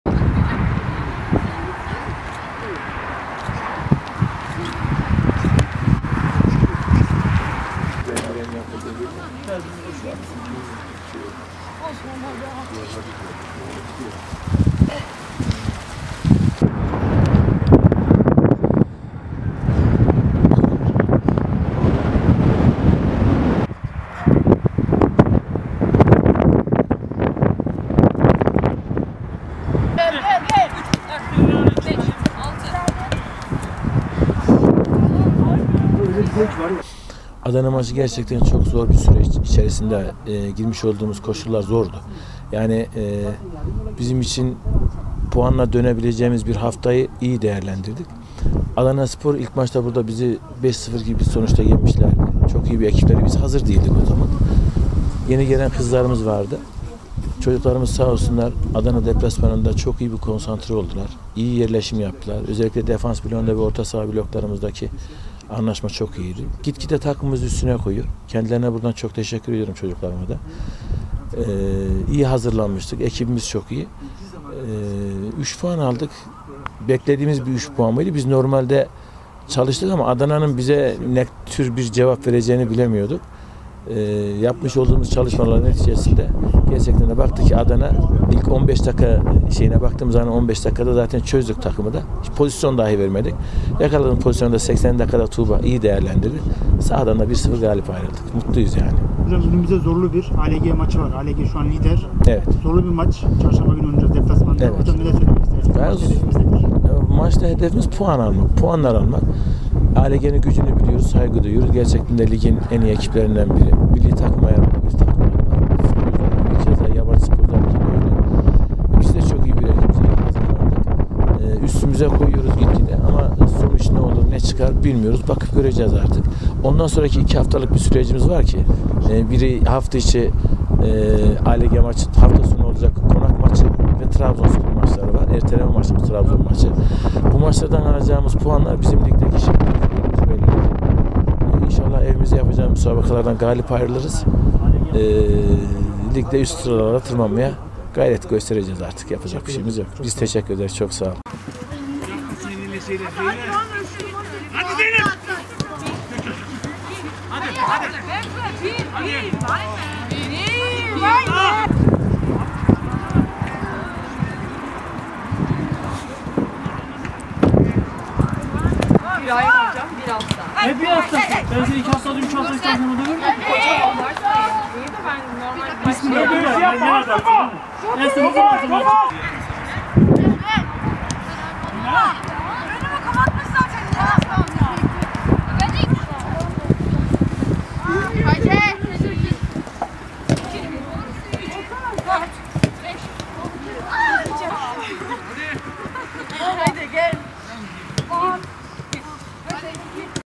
Sivasspor Kadın Futbol Takımı, gözünü lidere dikti!
Kadınlar Süper Ligi B Grubu’nda geçen hafta Adana İdmanyurdu’nu deplasmanda 1-0 yenerek çıkışa geçen Sivasspor Kadın Futbol Takımı gözünü lider ALG Spor maçına çevirdi.